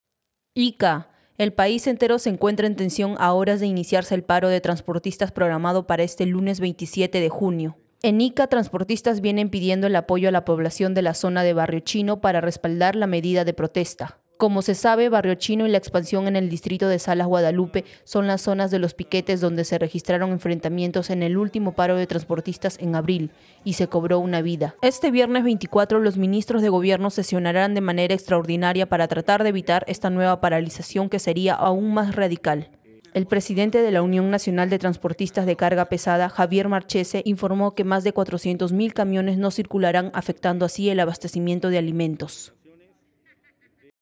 Piden apoyo a los pobladores de Barrio Chino para el Paro de Transportistas
El país entero se encuentra en tensión a horas de iniciarse el paro de transportistas programado para este lunes 27 de junio. En Ica, transportistas vienen pidiendo el apoyo a la población de la zona de Barrio Chino para respaldar la medida de protesta.

Como se sabe Barrio Chino y La Expansión en el distrito de Salas Guadalupe, son las zonas de los piquetes donde se registraron enfrentamientos en el último paro de transportistas en abril. y se cobró una vida.

Este viernes 24, los ministros de gobierno sesionarán de manera extraordinaria para tratar de evitar esta nueva paralización que sería aún más radical. El presidente de la Unión Nacional de Transportistas de Carga Pesada (UNT) Javier Marchese informó que más de 400 mil camiones no circularán afectando así al abastecimiento de alimentos.

LALUPANoticias. El medio de comunicación digital del sur del Perú.